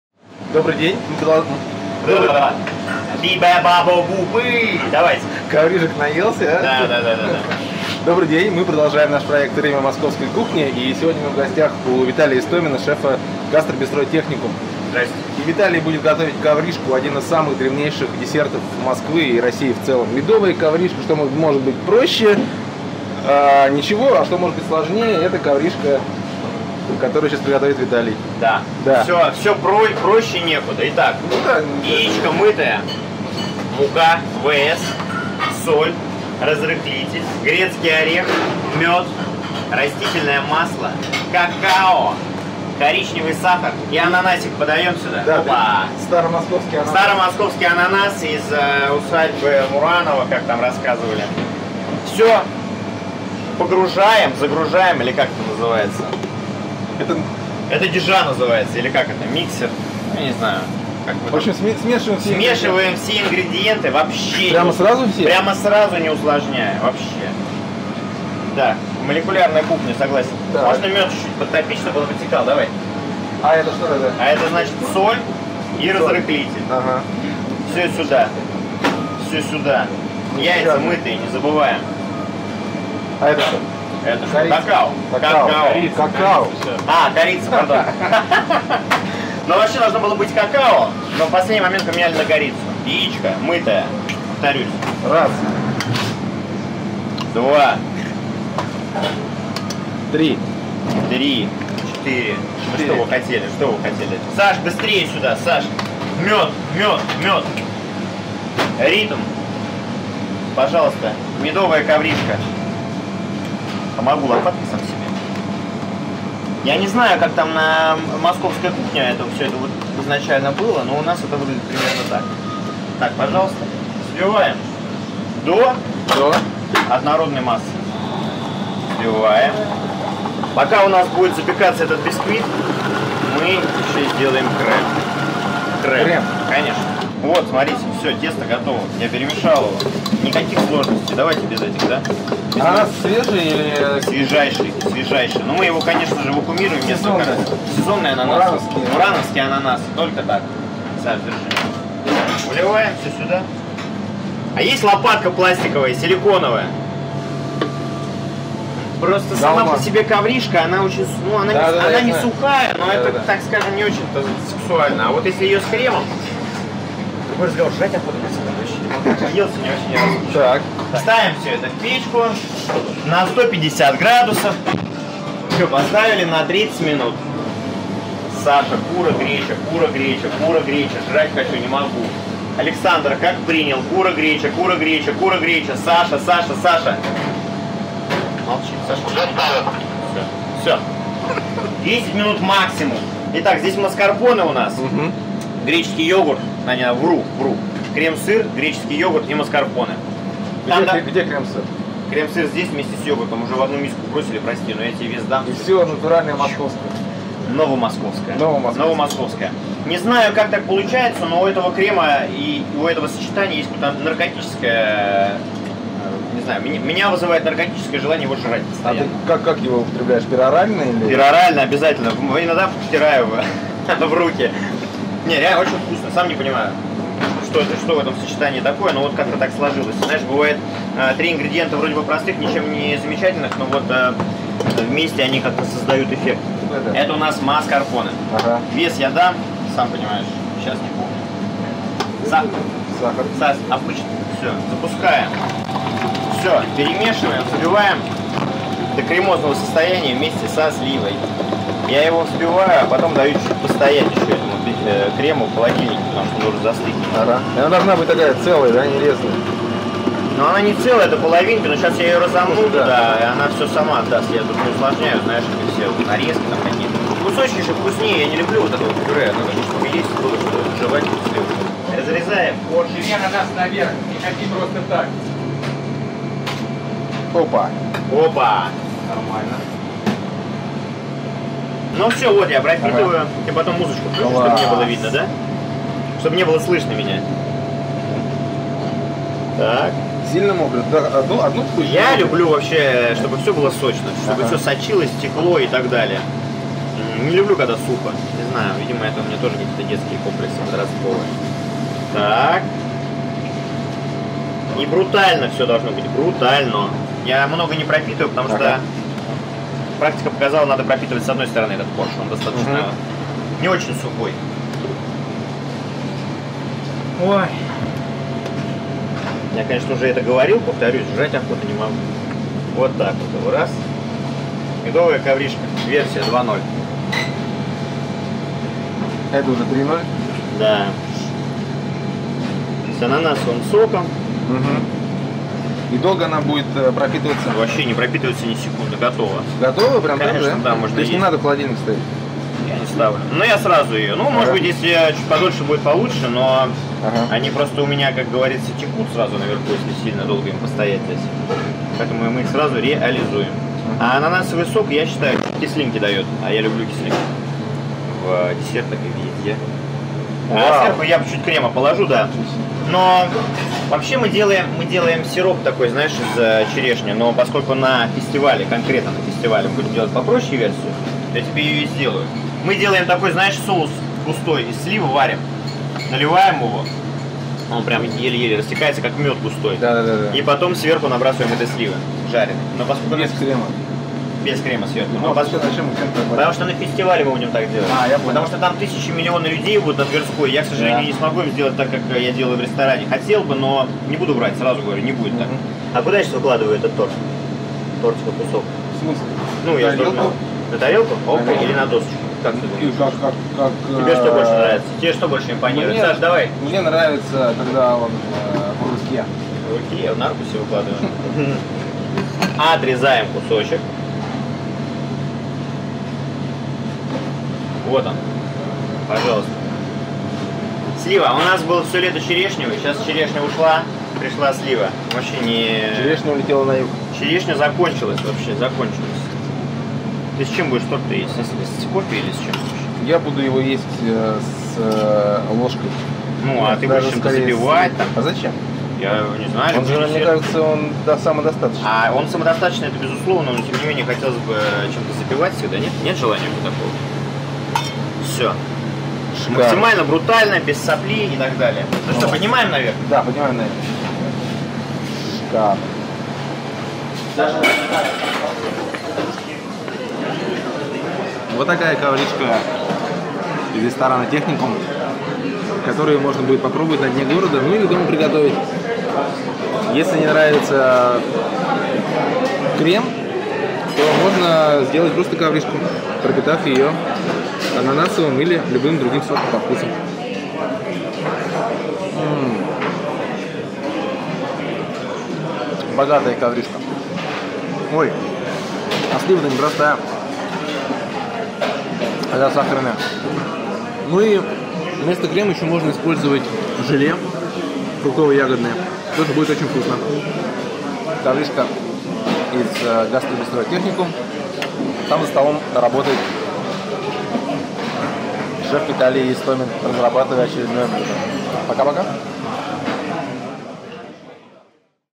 Коврижка
Медовая коврижка, еще одно блюдо из нашего проекта «Время московской кухни». Не простая, а богатая, со сливочно-сливовым кремом. Готовит очень голодный Виталий Истомин​, шеф гастробистро «Техникум»​. Главное — естественно, не коврижка, а крем, и дома можно готовить вообще без всего, а внутрь теста положить любые сочные фрукты по вкусу.